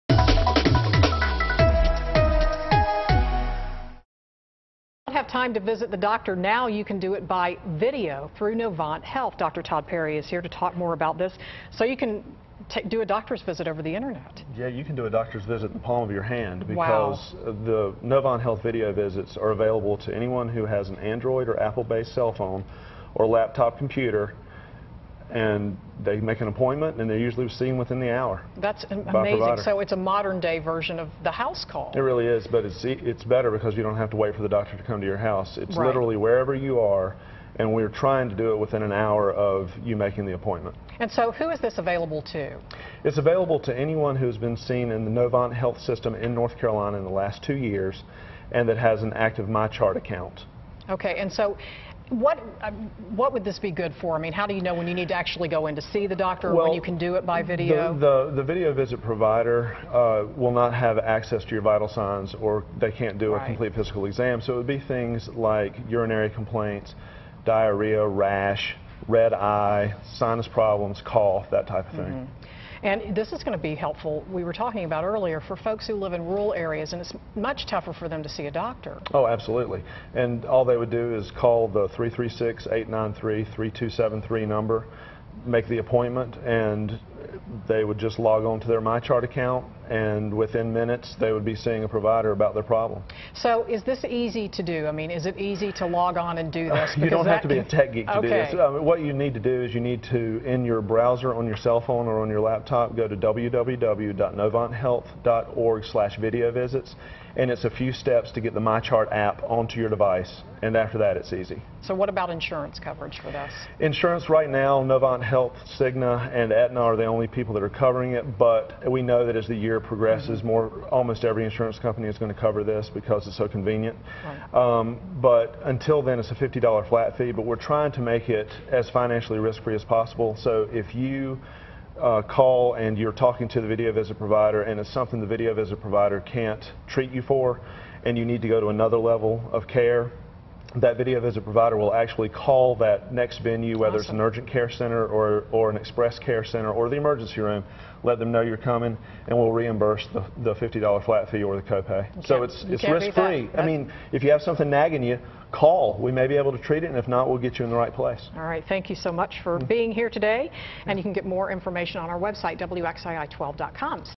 Doctor appointments over the Internet
Dr. Todd Perry of Novant Health Clemmons Express Care joins WXII 12's Kimberly Van Scoy during Thursday's noon newscast.